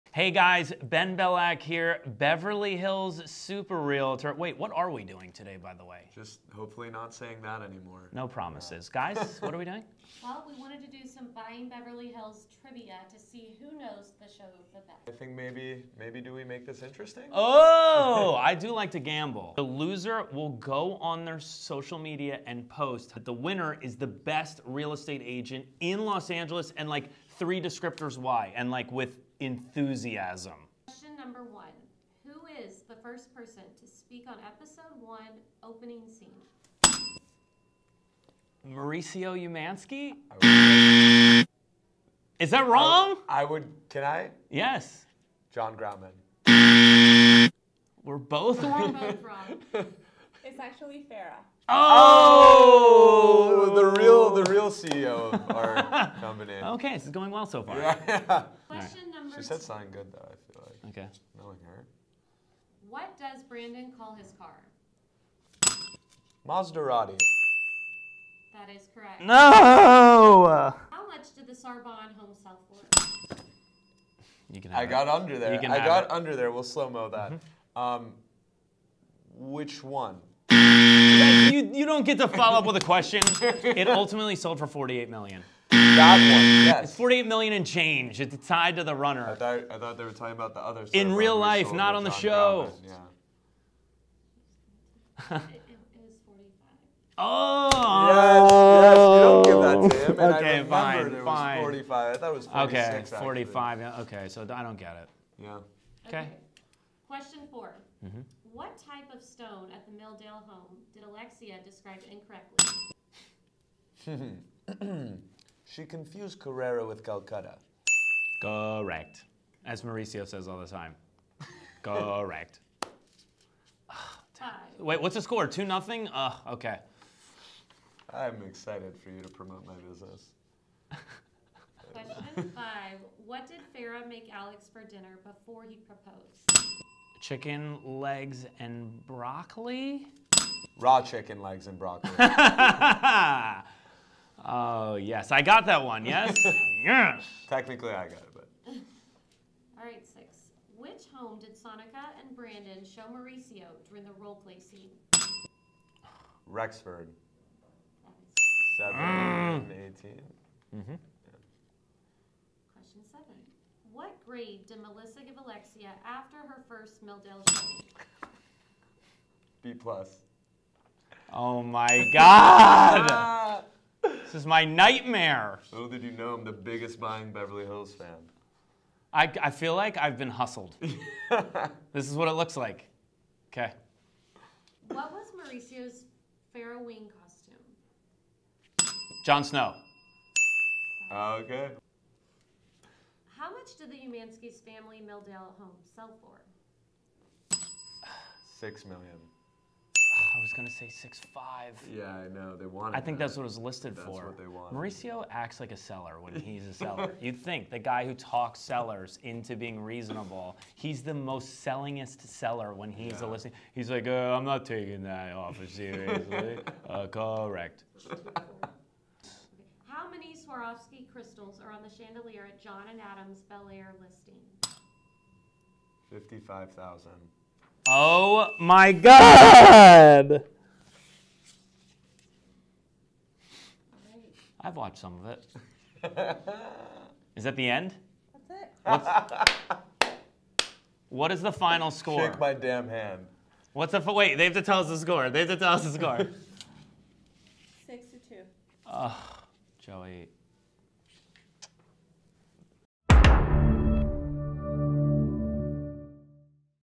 Buying Beverly Hills Trivia Battle
You won't believe what the loser has to do at the end of this epic @Netflix Buying Beverly Hills Season 1 trivia battle between cast members @benbelack and @joeybenzvi